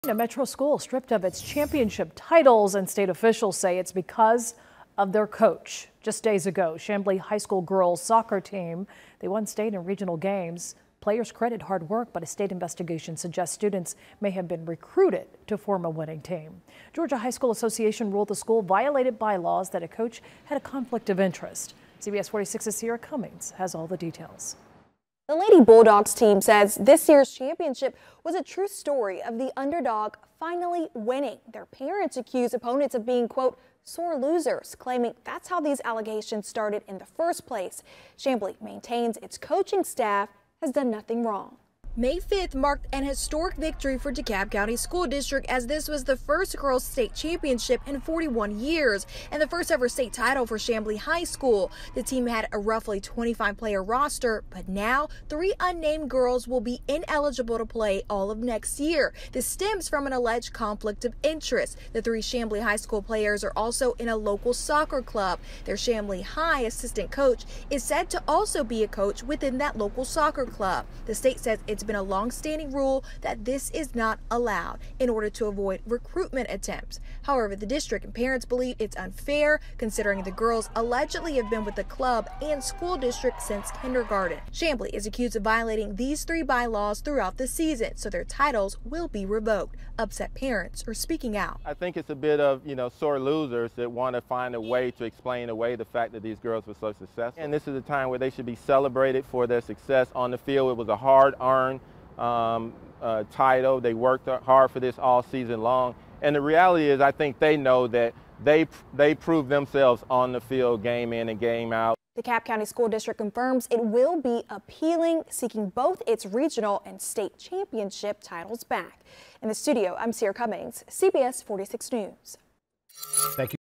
GHSA strips Chamblee High School girls soccer team of state and regional titles
The Georgia High School Association has stripped the Chamblee High School girls soccer team of all its victories during the 2021-22 season, including its regional and state championship because three by-laws were allegedly violated.